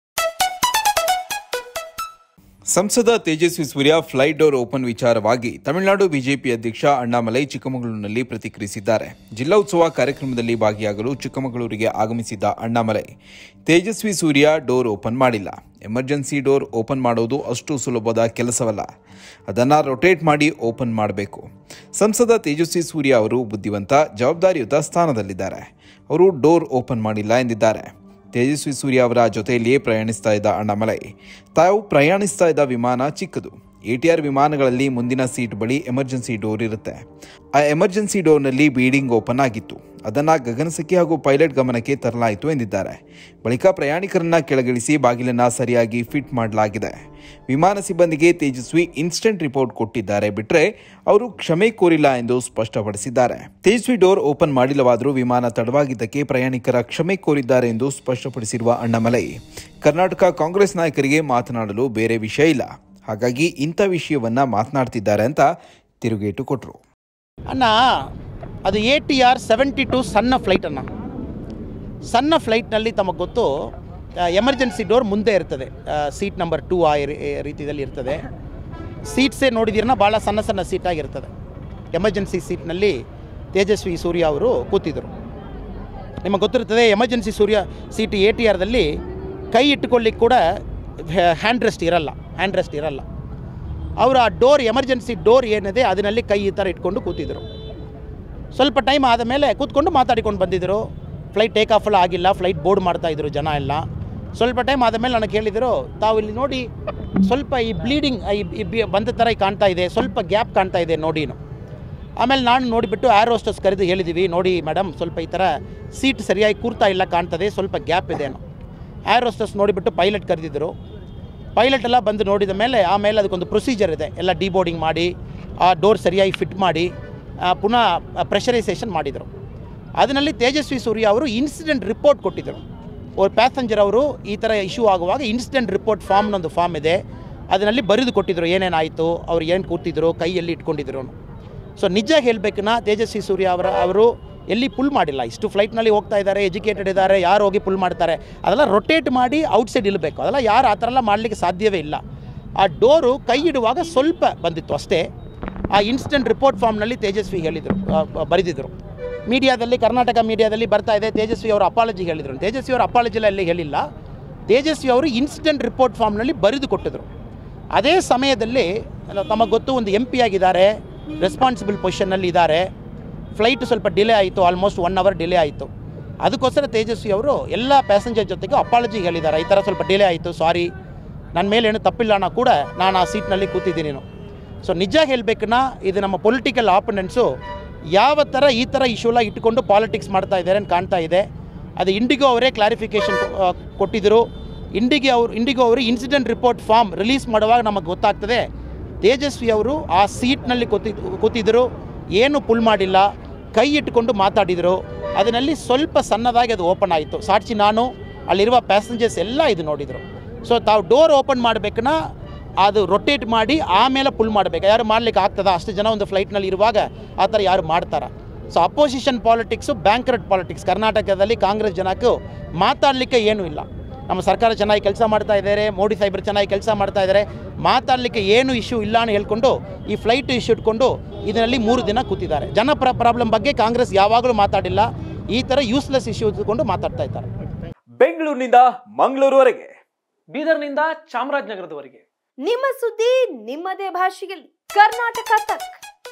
Tejasvi surya didn’t open emergency door nor apolagised, Annamalai tells in Chikkamagaluru
ಸಂಸದ ತೇಜಸ್ವಿ ಸೂರ್ಯ(Tejasvi surya) ಫ್ಲೈಟ್ ಡೋರ್ ಓಪನ್ ವಿಚಾರವಾಗಿ, ತಮಿಳುನಾಡು ಬಿಜೆಪಿ ಅಧ್ಯಕ್ಷ ಅಣ್ಣಾಮಲೈ(Annamalai) ಚಿಕ್ಕಮಗಳೂರಿನಲ್ಲಿ ಪ್ರತಿಕ್ರಿಯಿಸಿದ್ದಾರೆ. ಜಿಲ್ಲಾ ಉತ್ಸವ ಕಾರ್ಯಕ್ರಮದಲ್ಲಿ ಭಾಗಿಯಾಗಲು ಚಿಕ್ಕಮಗಳೂರಿಗೆ ಆಗಮಿಸಿದ್ದ ಅಣ್ಣಾಮಲೈ, ತೇಜಸ್ವಿ ಸೂರ್ಯ ಡೋರ್ ಓಪನ್ ಮಾಡಿಲ್ಲ, ಎಮರ್ಜೆನ್ಸಿ ಡೋರ್(Emergency Door) ಓಪನ್ ಮಾಡುವುದು ಅಷ್ಟು ಸುಲಭದ ಕೆಲಸವಲ್ಲ. ಅದನ್ನ ರೊಟೇಟ್ ಮಾಡಿ ಓಪನ್ ಮಾಡಬೇಕು ಎಂದಿದ್ದಾರೆ.  ಸಂಸದ ತೇಜಸ್ವಿ ಸೂರ್ಯ ಅವರು ಬುದ್ಧಿವಂತ, ಜವಾಬ್ದಾರಿ ಸ್ಥಾನದಲ್ಲಿದ್ದಾರೆ ಅವರು ಡೋರ್ ಓಪನ್ ಮಾಡಿಲ್ಲ ಎಂದಿದ್ದಾರೆ. ತೇಜಸ್ವಿ ಸೂರ್ಯ ಅವರ ಜತೆಯಲ್ಲಿಯೇ ಪ್ರಯಾಣಿಸುತ್ತಿದ್ದ ಅಣ್ಣಾಮಲೈ, ತಾವು ಪ್ರಯಾಣಿಸುತ್ತಿದ್ದ ವಿಮಾನ ಚಿಕ್ಕದು. ಎಟಿಆರ್ ವಿಮಾನಗಳಲ್ಲಿ ಮುಂದಿನ ಸೀಟಿನ ಬಳಿ ಎಮರ್ಜೆನ್ಸಿ ಡೋರ್ ಇರುತ್ತೆ. ಆ ಎಕ್ಸಿಟ್ ಡೋರ್ ನಲ್ಲಿ ಬೀಡಿಂಗ್ ಓಪನ್ ಆಗಿತ್ತು. ಅದನ್ನ ಗಗನಸಖಿ ಹಾಗೂ ಪೈಲಟ್ ಗಮನಕ್ಕೆ ತರಲಾಯಿತು ಎಂದಿದ್ದಾರೆ. ಬಳಿಕ ಪ್ರಯಾಣಿಕರನ್ನ ಕೆಳಗಿಸಿ ಬಾಗಿಲನ್ನ ಸರಿಯಾಗಿ ಫಿಟ್ ಮಾಡಲಾಗಿದೆ ಎಂದಿದ್ದಾರೆ. ವಿಮಾನ ಸಿಬ್ಬಂದಿಗೆ ತೇಜಸ್ವಿ ಇನ್ಸ್ ಟೆಂಟ್ ರಿಪೋರ್ಟ್ ಕೊಟ್ಟಿದ್ದಾರೆ ಬಿಟ್ಟರೆ ಅವರು ಕ್ಷಮೆ ಕೋರಿಲ್ಲ ಎಂದು ಸ್ಪಷ್ಟಪಡಿಸಿದ್ದಾರೆ.ತೇಜಸ್ವಿ ಡೋರ್ ಓಪನ್ ಮಾಡಿಲ್ಲವಾದ್ರೂ, ವಿಮಾನ ತಡವಾಗಿದ್ದಕ್ಕೆ ಪ್ರಯಾಣಿಕರ ಕ್ಷಮೆ ಕೋರಿದ್ದಾರೆ ಎಂದು ಸ್ಪಷ್ಟಪಡಿಸಿದ್ದಾರೆ. ಕರ್ನಾಟಕ ಕಾಂಗ್ರೆಸ್‍ ನಾಯಕರಿಗೆ ಮಾತನಾಡಲು ಬೇರೆ ವಿಷಯ ಇಲ್ಲ, ಹಾಗಾಗಿ, ಇಂತಹಾ ವಿಷಯವನ್ನ ಮಾತನಾಡ್ತಿದ್ದಾರೆ ಎಂದು ತಿರುಗೇಟು ಕೊಟ್ಟಿದ್ದಾರೆ.

After the Filght Emergency Door Open incident made headlines, Tejasvi Surya, Bengaluru South MP and BJP Yuva Morcha National President, was targeted. Surya's co-passenger, Tamilnadu BJP President Annamalai, has spoken out about the incident in Chikkamagalur. Annamalai clarified that Tejasvi Surya did not open the emergency door. The ATR-72 is a small plane, and the emergency seat will be on the front. When we boarded the plane and took our seats, Surya noticed that there was a small gap in the emergency exit door. We brought it to the air hostess and pilot’s notice. After that, passengers were deboarded and the door was locked properly according to the procedure. Annamalai questioned why Tejasvi, an intelligent and responsible MP, would open the fight door. Tejasvi has filled out the instant report form for the flight about the incident. Annamalai clarified that he had not sent an apology letter.